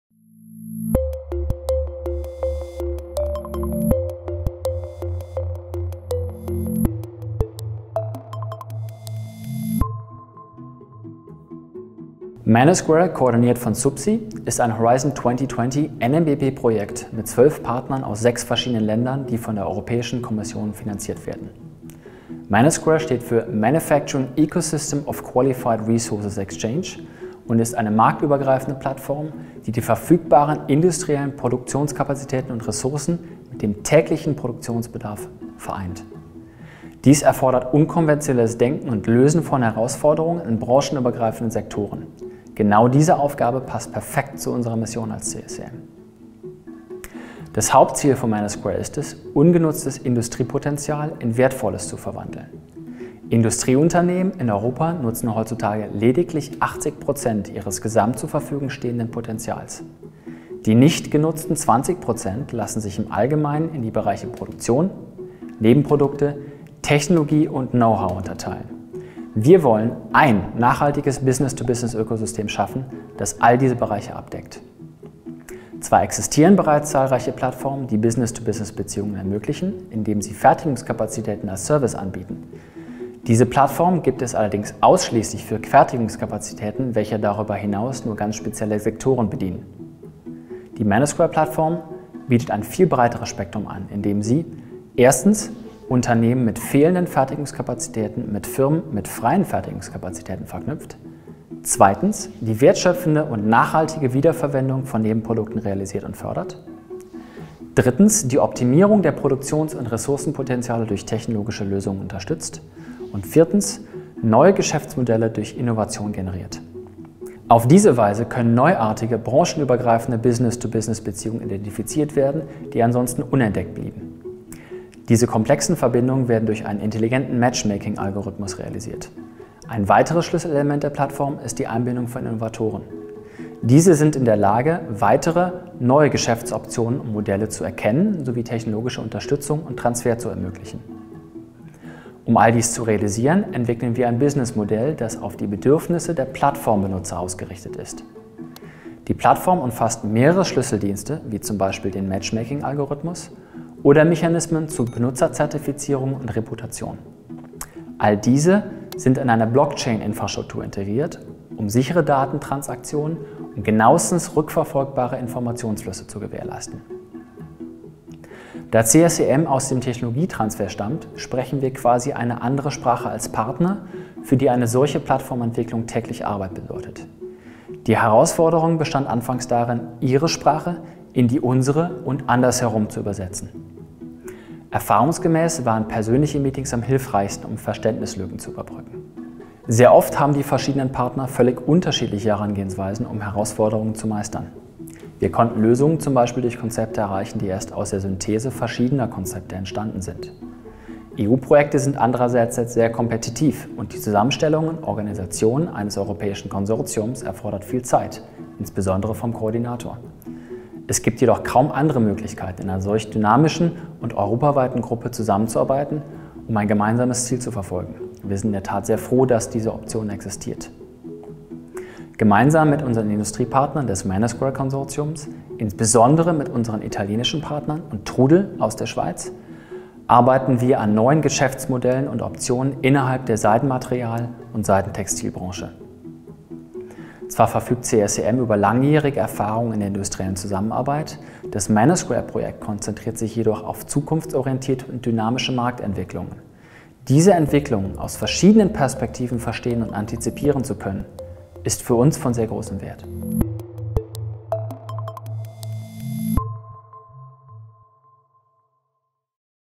MANUSQUARE - MANUfacturing ecoSystem of QUAlified Resources Exchange
MANUSQUARE, koordiniert von SUPSI, ist ein Horizon 2020 NMBP-Projekt. 12 Partnern aus 6 verschiedenen Ländern (inkl. CSEM Landquart, Schweiz) sind von der Europäischen Kommission finanziert. MANUSQUARE steht für MANUfacturing ecoSystem of QUAlified Resources Exchange. Es ist eine marktübergreifende Plattform, die die verfügbaren, industriellen Produktionskapazitäten und -ressourcen mit dem tatsächlichen Produktionsbedarf vereint. Dies erfordert unkonventionelles Denken und Lösen von Herausforderungen in branchenübergreifenden Sektoren.